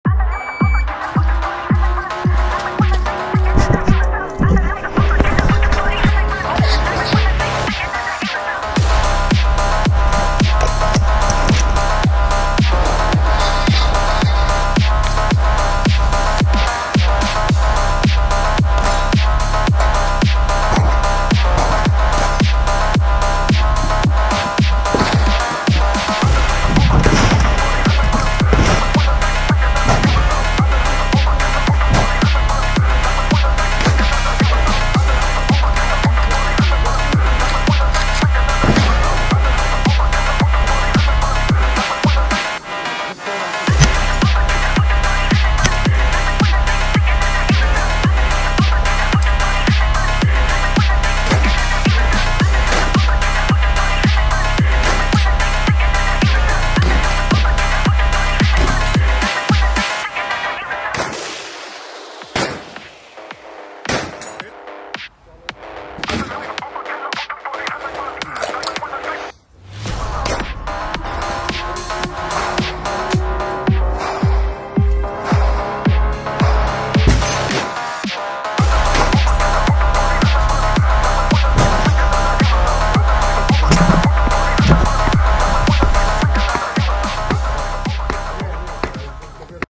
EXPERT TACTICAL BATTLEGROUND COLD EDITION VOL.1
Skrót z ostatniej edycji zawodów taktyczno-strzeleckich Expert Tactical Battleground w Jaworznie.